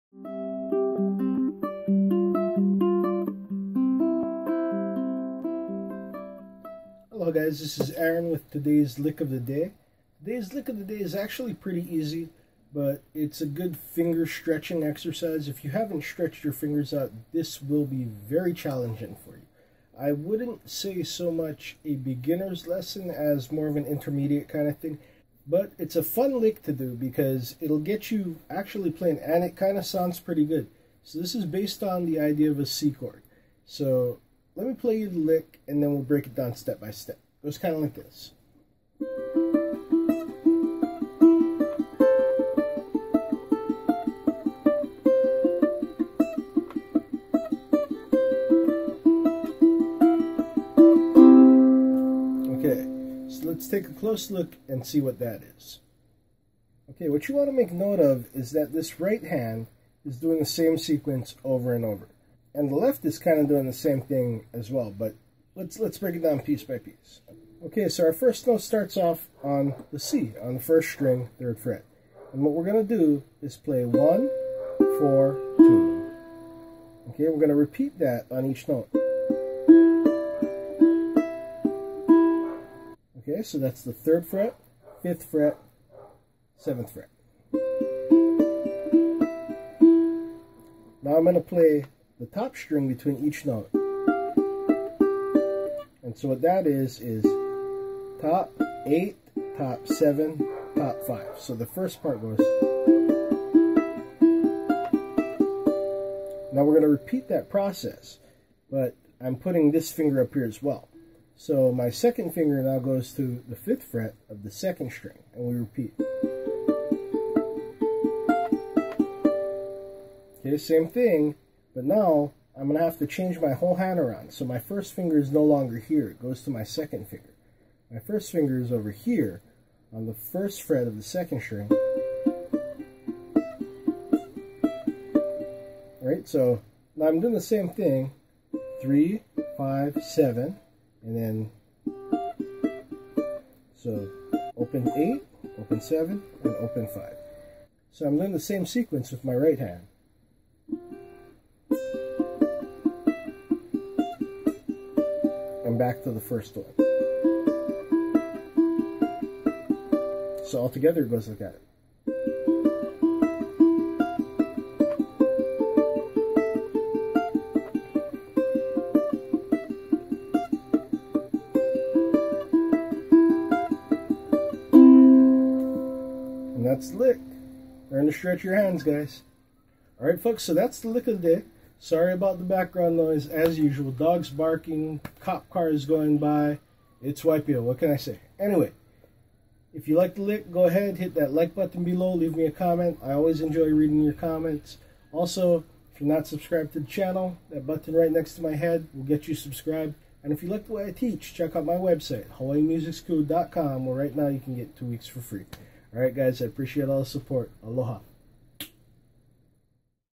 Ukulele Lesson for LICKoftheDAY 08/06/18
Here's a fun lick to get your left and right hand in synchronization with each other. Also great stretching exercise for the fretting hand! I'm calling this an intermediate level lick due to the stretching only... beginners with large hands and/or good dexterity should have little to no problems as well!

If you like the lesson and want to learn more, check out for a FREE 2 week trial membership!